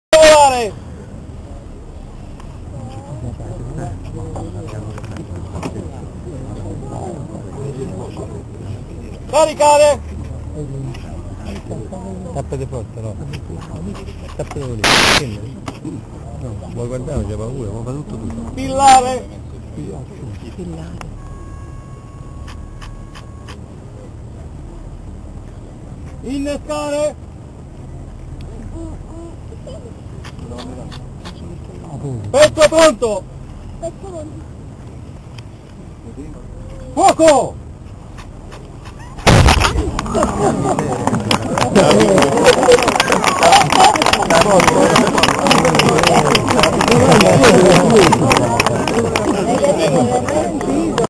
XV Congresso Gaeta Due Sicilie Maria Sofia Borbone
Gaeta 2007, là dove sorgeva la Transilvania, salve  di cannone per commemorare gli Eroici Difensori delle Due Sicilie, e tutti i caduti Napoletani e Siciliani del 1860-61.